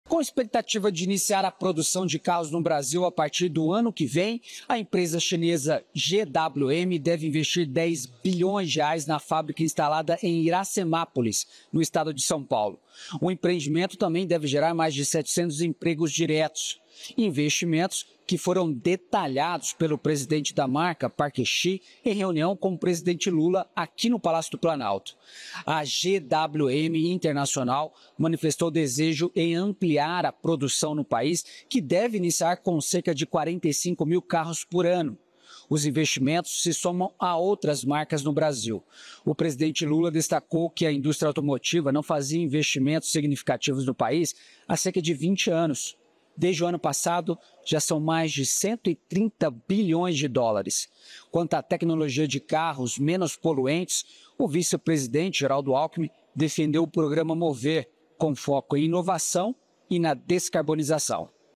Lula é convidado para participar da inauguração da multinacional chinesa GWM em SP
O presidente Lula recebeu do presidente da multinacional chinesa GWM o convite para participar da inauguração da fábrica no Brasil. Com investimento de R$ 10 bilhões, a estimativa é de que a unidade da empresa chinesa em Iracemápolis (SP) comece a operar em 2025 e gere, inicialmente, 700 empregos diretos.